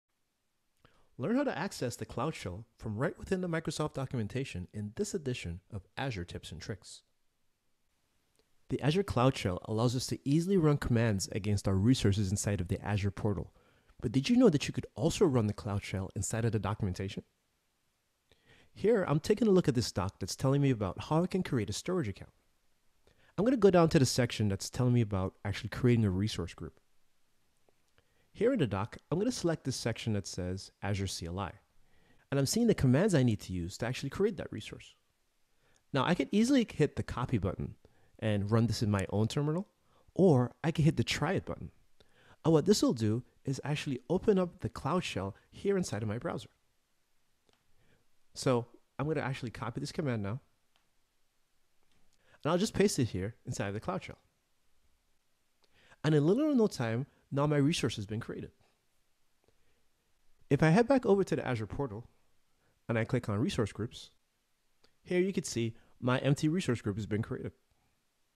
How to access Cloud Shell from within Microsoft docs | Azure Tips and Tricks
In this edition of Azure Tips and Tricks, you'll learn how to quickly access Azure Cloud Shell from within the Microsoft documentation. The Azure Cloud Shell allows us to easily run commands against our resources inside of the Azure portal. But did you know that you can easily run the Cloud Shell inside of the documentation? Watch to learn how.

Get started with 12 months of free services and $200 USD in credit. Create your free account today with Microsoft Azure